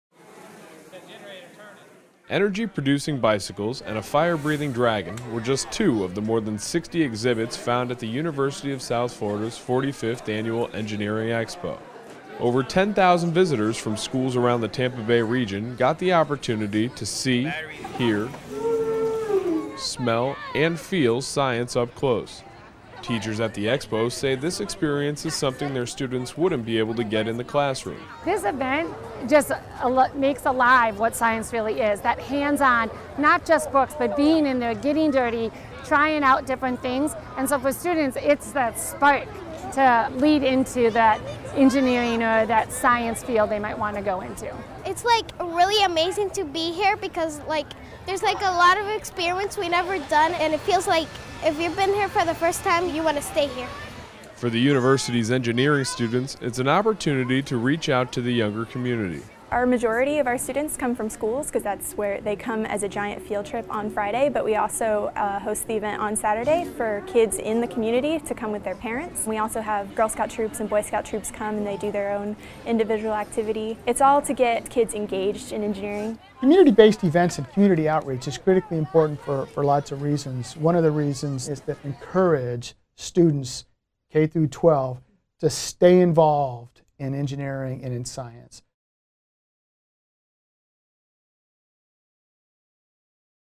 USF Engineering Expo 2017
The University of South Florida's Engineering Expo is a two-day student-run event that has elementary, middle and high school students from across the Tampa Bay region experiencing hands-on exhibits that act as examples for all things physics, chemistry and engineering.

Produced by Ryan Noone.